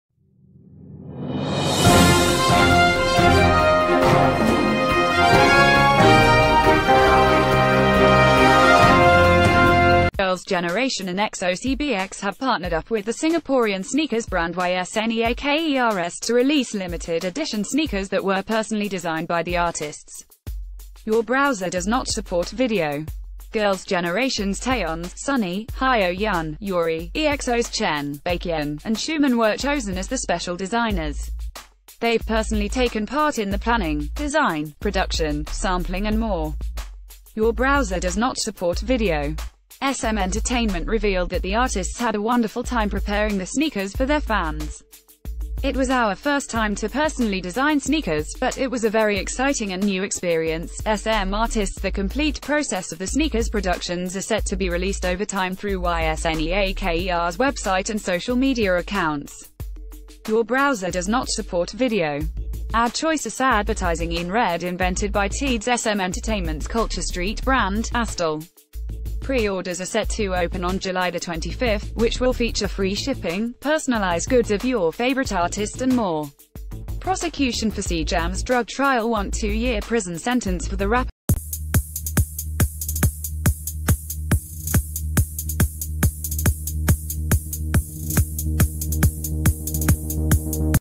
Girls Generation And EXO-CBX To Release Personally Designed Limited Edition Sneakers
Girls’ Generation and EXO-CBX have partnered up with the Singaporean sneakers brand YSNEAKERS to rel...